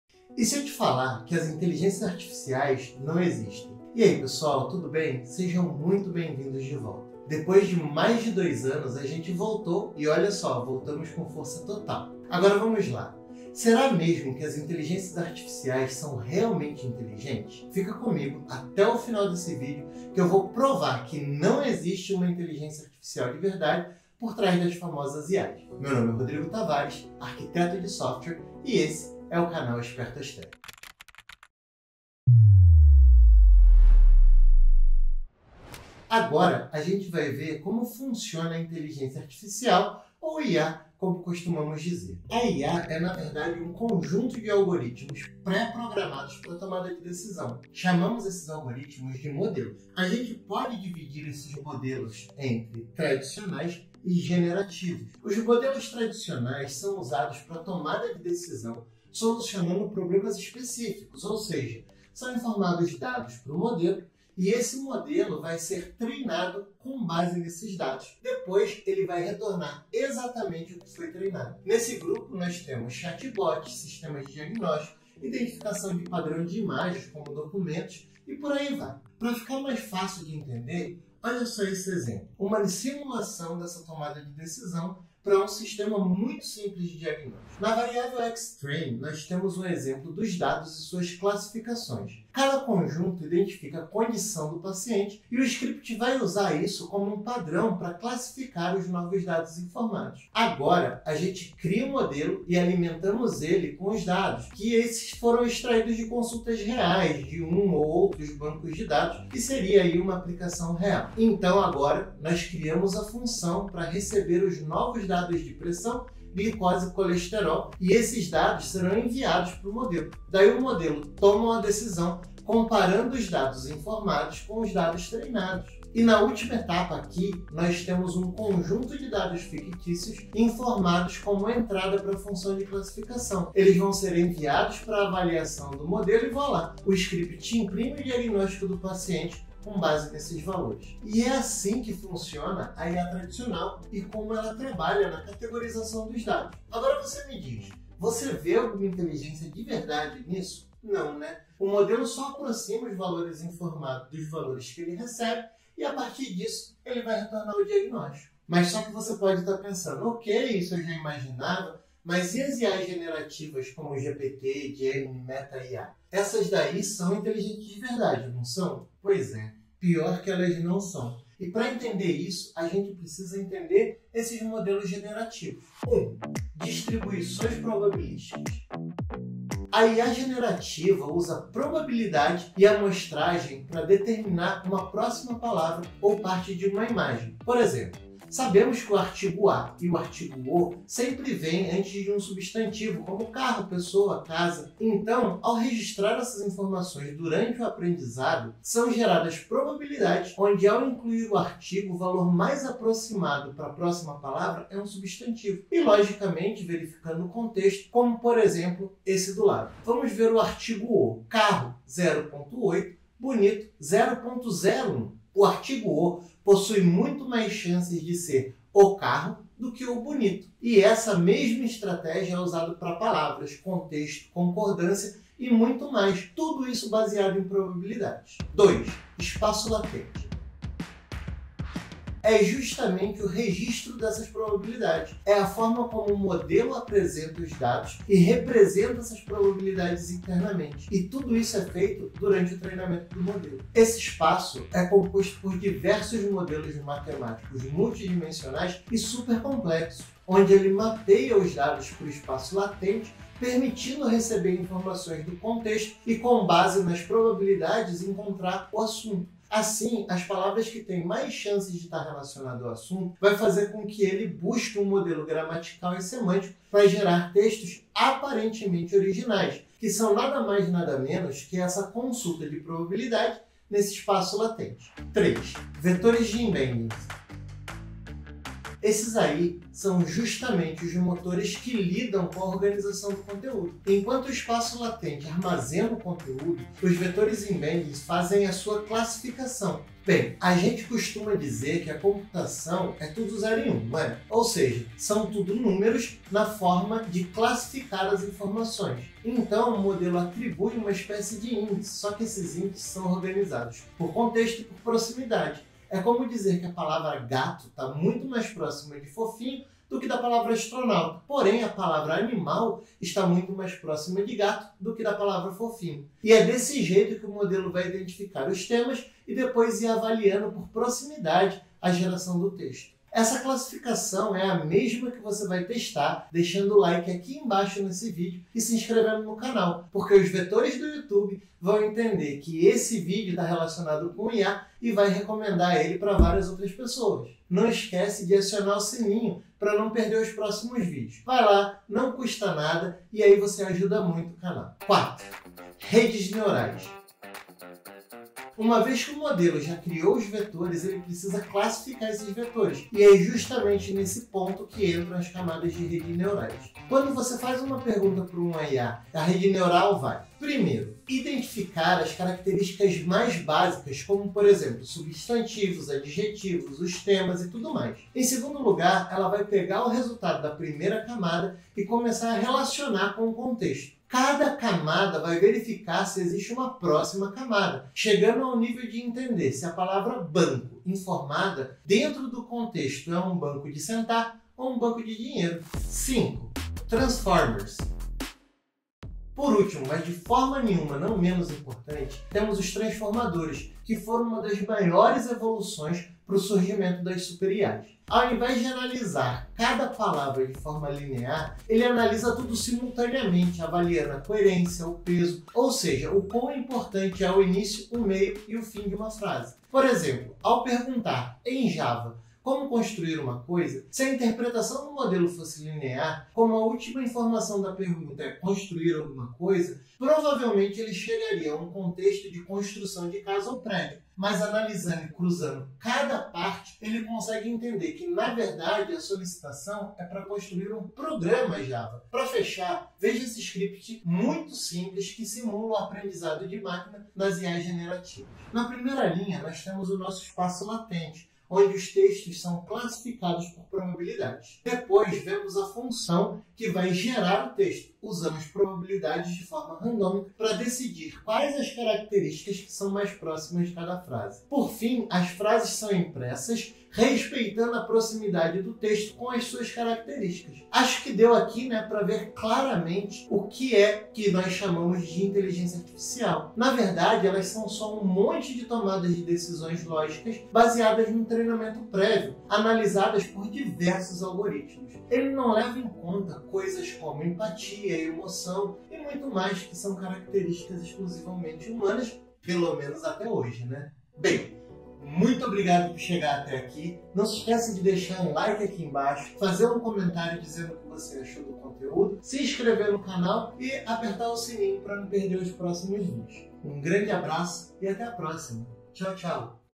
Inteligência Artificial (IA) NÃO Existe!
Como funcionam as "Inteligências Artificiais" (IAs)? Por que não são inteligentes? Entenda de uma vez por todas as Inteligências Artificiais Tradicionais e Generativas como Chat GPT, Gemini e Meta AI. Aprenda sobre como as IAs usam Distribuições Probabilísticas, Espaço Latente, Vetores de embeddings, Redes Neurais e Transformers.
Com códigos em Python simulando essas Inteligências Artificiais, você vai entender de uma vez por todas por que as IAs NÃO Existem!

00:00 Introdução
00:55 IA Tradicional
02:38 IA Generativa
07:40 Conclusão